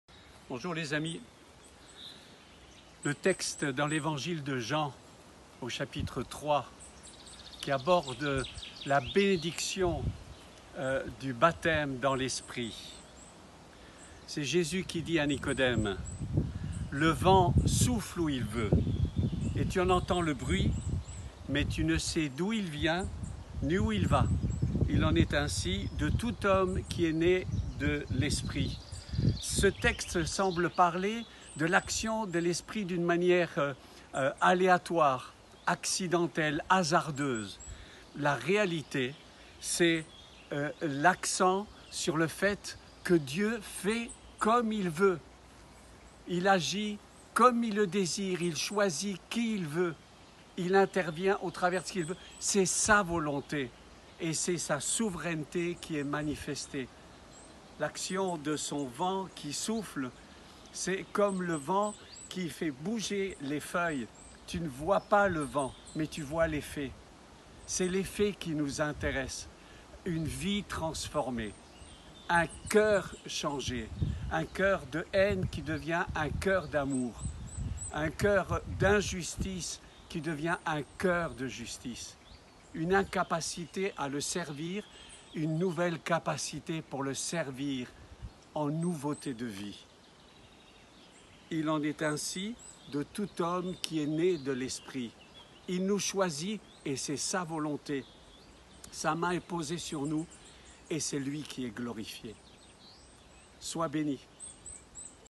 2 Minutes avec Jésus - Vincent Fernandez
Verset du jour
Jean 3:8 Le vent souffle où il veut, et tu en entends le bruit; mais tu ne sais d'où il vient, ni où il va. Il en est ainsi de tout homme qui est né de l'Esprit.
Caractéristique du baptême dans l’Esprit, être conduit par Lui...
Soyez bénis !